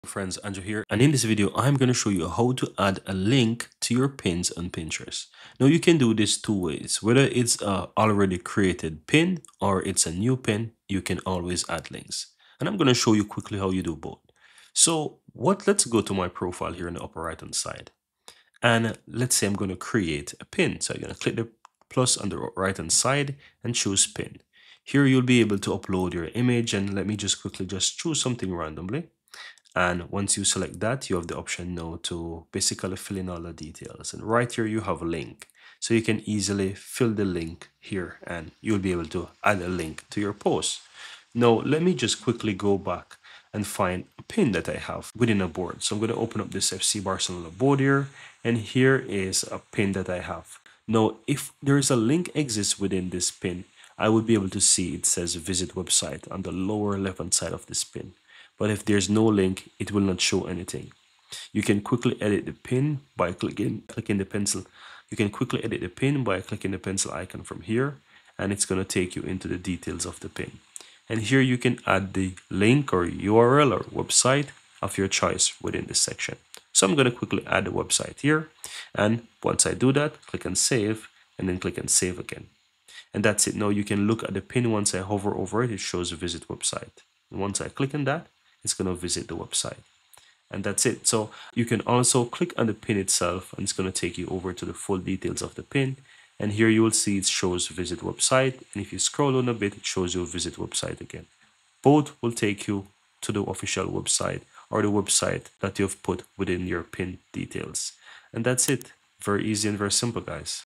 How to Add a Clickable Link to ANY Pinterest Pin!
Want to drive more traffic to your website or blog from Pinterest? In this tutorial, we'll show you a simple trick to add a clickable link to ANY Pinterest pin! Whether you're a blogger, marketer, or entrepreneur, this hack will help you increase your online visibility and reach a wider audience. Learn how to add a clickable link to your Pinterest pins and start driving more traffic to your website today!